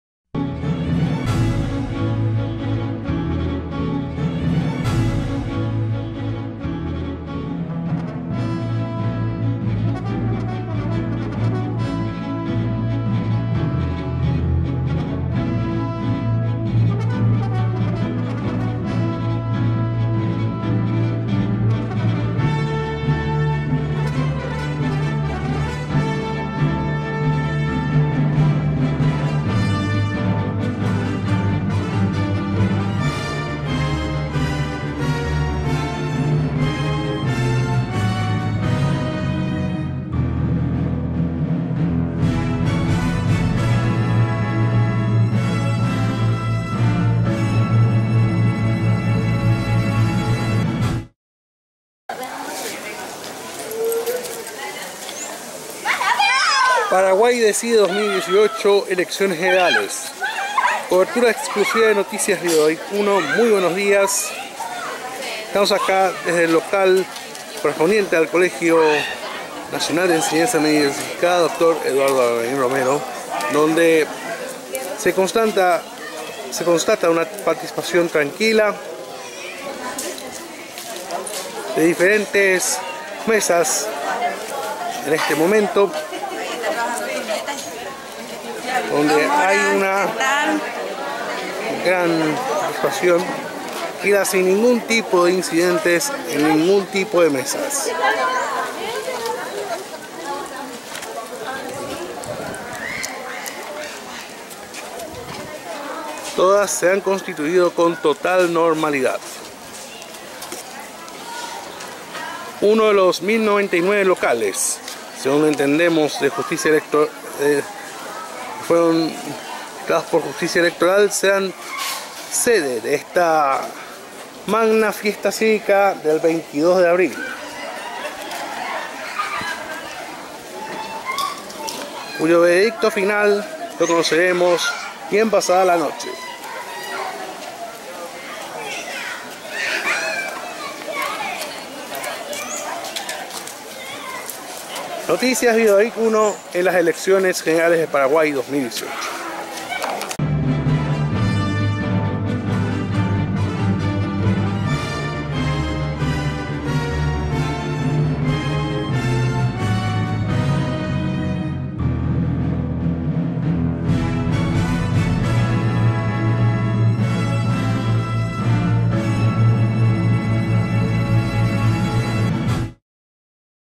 PARAGUAY DECIDE 2018: Tranquilidad en el Alvarín Romero
ParaguayDecide2018: Tranquilidad en el Alvarín Romero
Esta mañana, se observó una tranquilidad absoluta en el local de votación el Barrio Villa Morra.